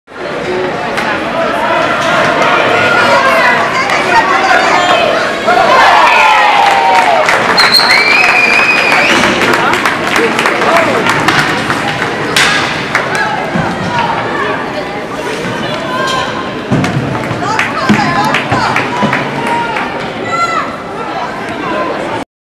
But samuel Berthelot, Vict. 10-4 vs Témiscouata, Chandler, 5 nov. 16.
Samuel Berthelot (12) entre avec vitesse en zone adverse en déjouant le premier défenseur avec une belle feinte, et marque d'un lancer précis à la droite du gardien du Témiscouata.